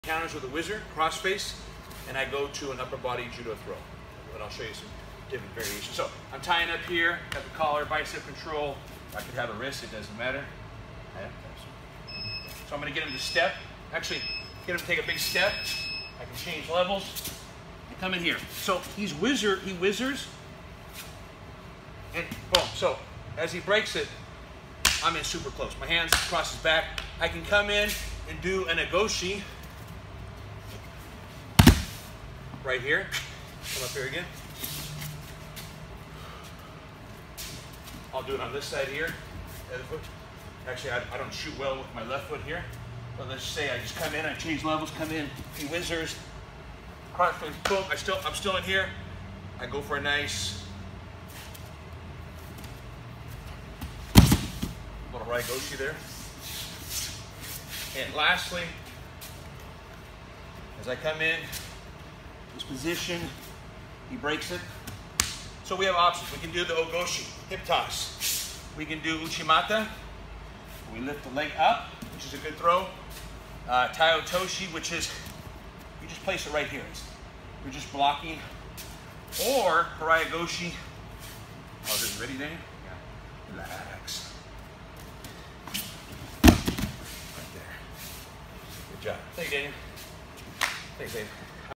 Wrestling Hip Toss
Wrestling single leg to hip toss—O goshi Judo.  Body Locks. Upper body wrestling throws.